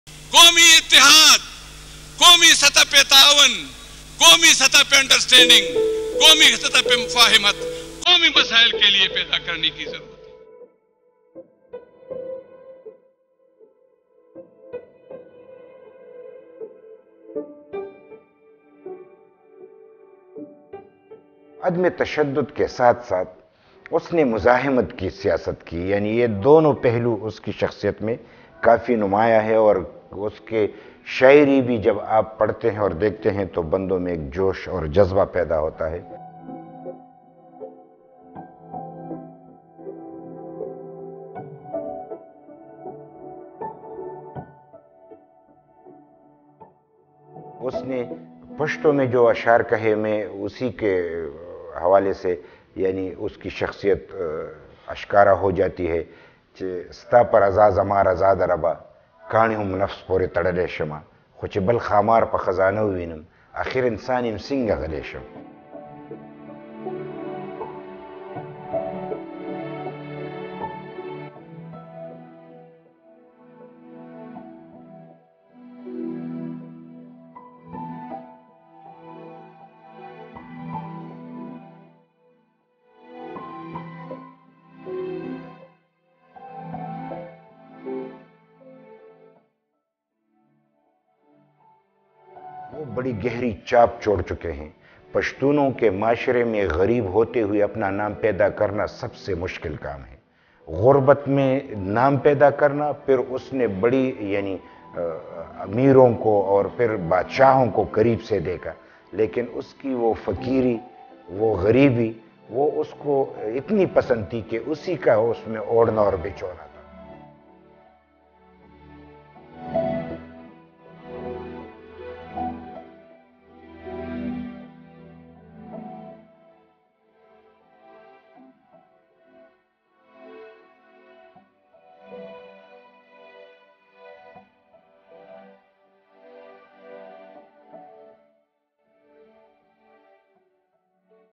Ajmal Khattak - The symbol of resistance
A short biographical video about AjmalKhattak on his 9th death anniversary, produced by AMN TV.